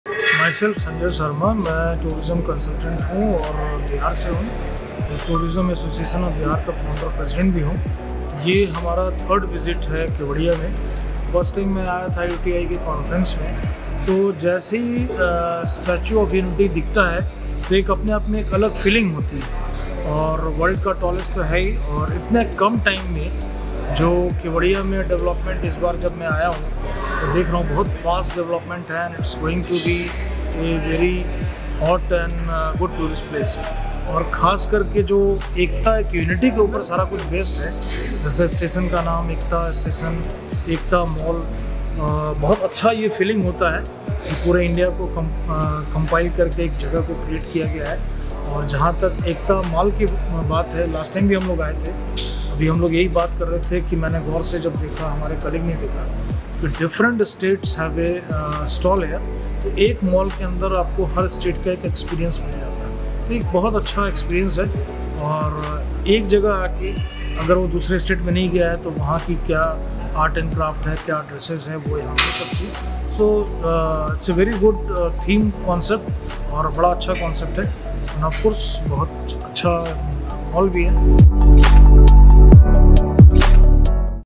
Sanjay Sharma, a visitor from Bihar, expresses his enthusiasm after a visit to the StatueOfUnity.
Sanjay Sharma, a visitor from Bihar, expresses his enthusiasm after a visit to the StatueOfUnity. He particularly highlights Ekta Mall, describing it as a unique destination that beautifully showcases a diverse array of offerings from every state, providing visitors with a glimpse of the rich culture of our nation.